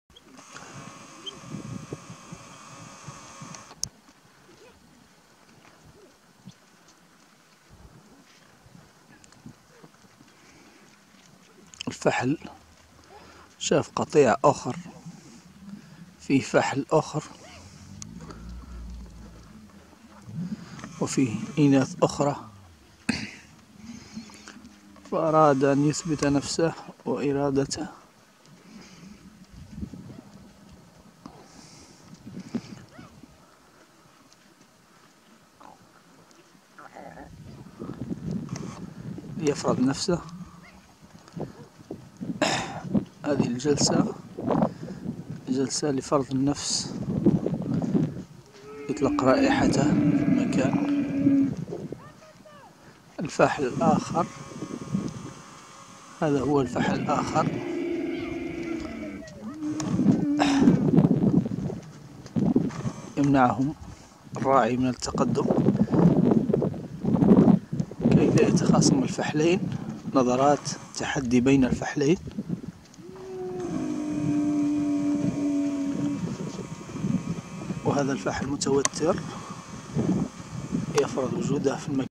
هيبة وقوة الجمل المسيطر/لن تصدق مايفعله فحل الجمل ليبعد الذكور الأخرى
أغراء الأناث لذكر المسيطر وستعدادهم لتزاوج
هيبة وقوة الجمل المسيطر

الكاميرا المستعمله
sony hdr-cx405
موقعنا الرسمي
غوغل +